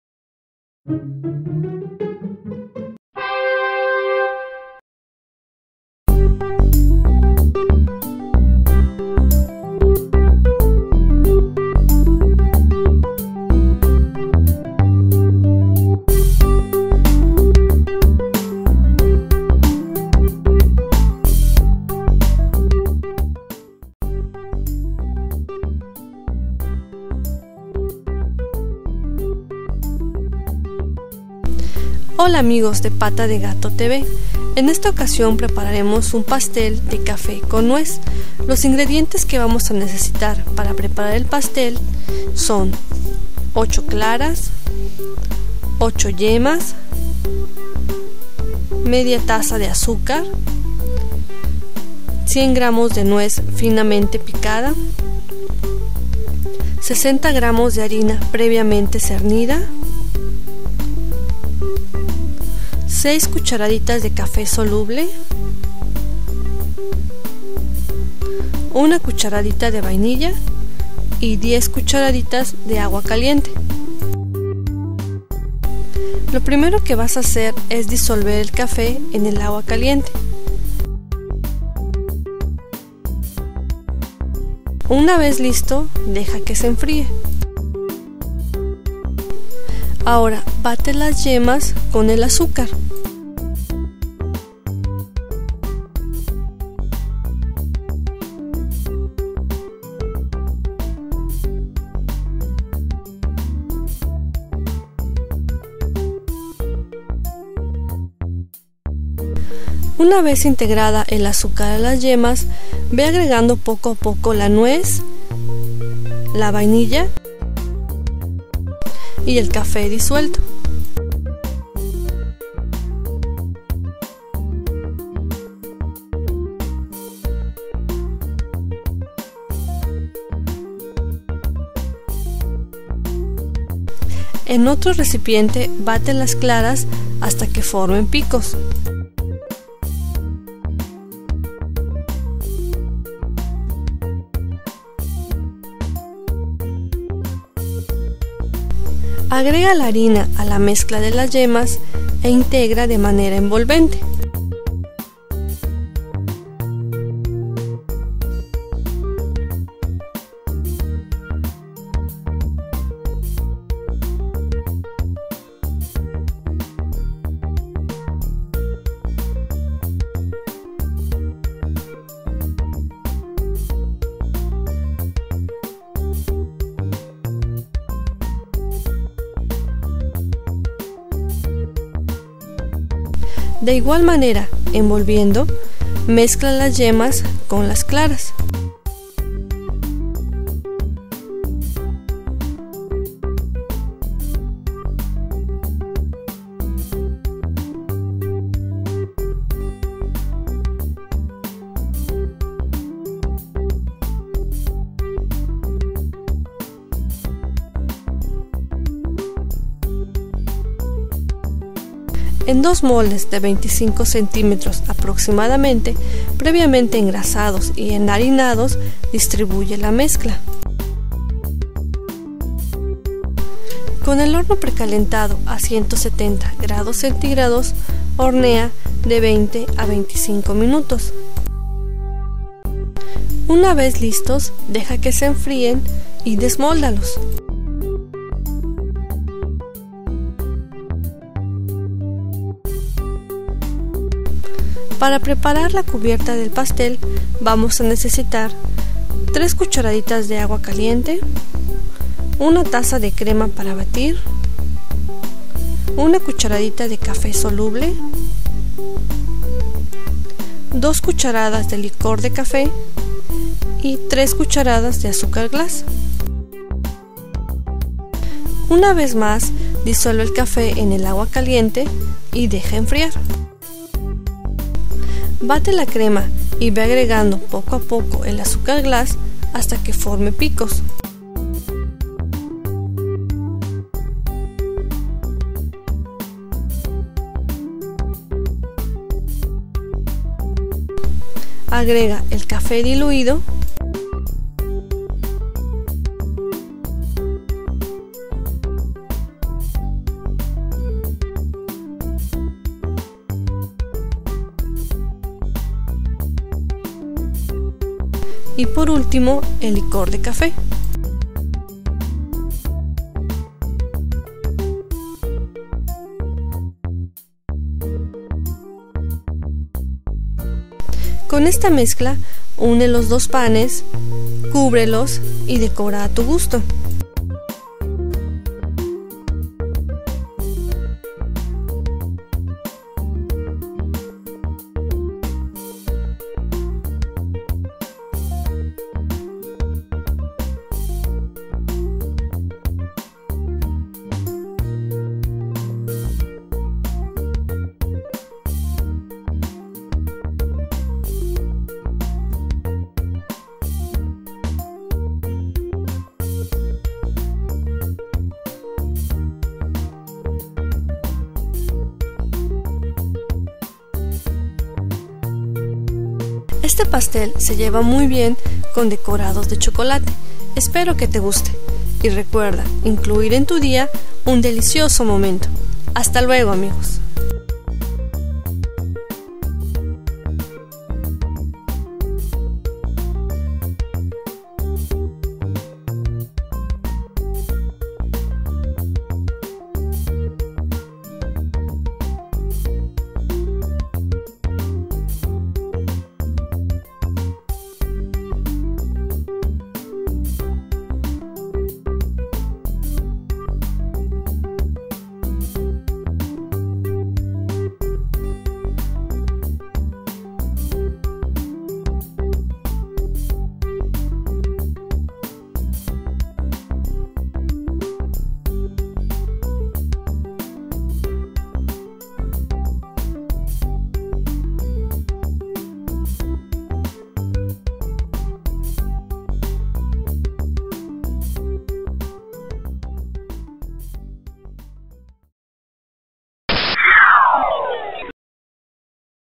Pastel de Café y Nuez / Coffee and Pecan Cake
Delicioso y elegante pastel cubierto y relleno con crema sabor café y decoración de chocolate / Delicious and elegant cake covered and stuffed with coffee cream and chocolate decorations (English subtitles)

¡Conoce todos los videos de Cocinando con Adriana de Pata de Gato TV!

¡Conóce nuestro canal! ¡Tenemos otros programas que te podrían gustar!